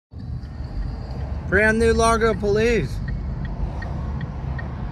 Newest Largo Police SUVs In Black
I Talked To 2 Largo Cops Today They Were Eating Lunch Where i Was At They Said All The Largo Police Cars Will Be That Black Soon